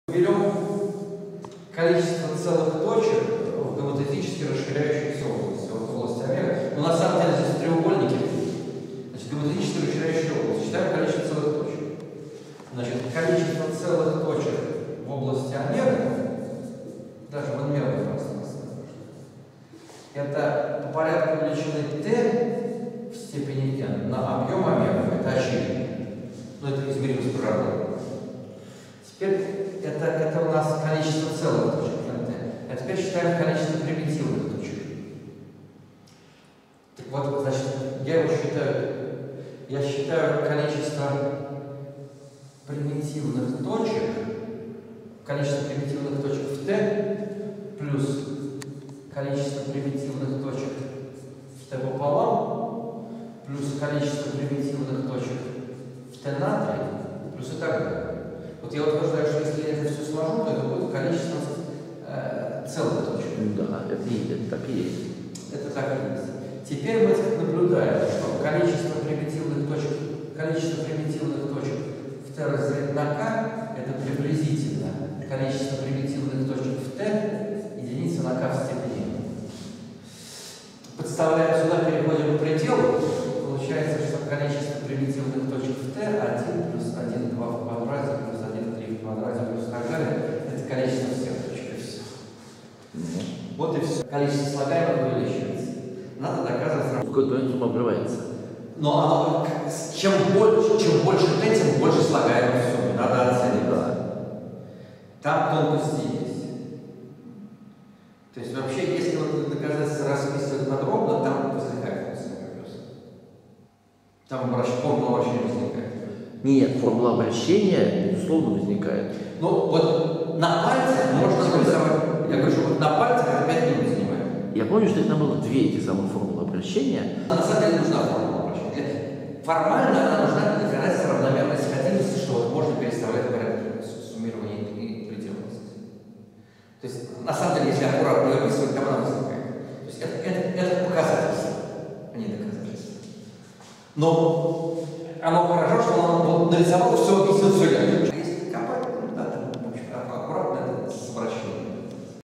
18 Идея другого способа
Лекция профессора кафедры теории чисел мехмата МГУ Николая Германовича Мощевитина на летней школе А.В. Спивака в университете Авейру (Португалия).